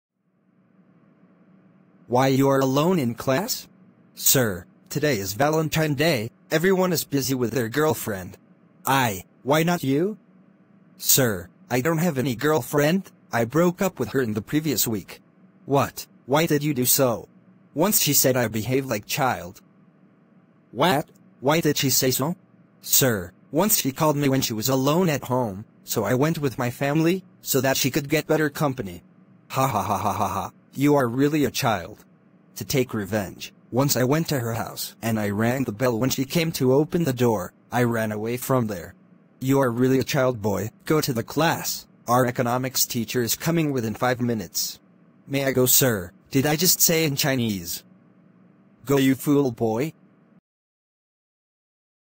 when you behave childish infront of girlfriend | wow amazing
This video is all about a boy studying in college.he had a girlfriend who left him because of his childish behaviour.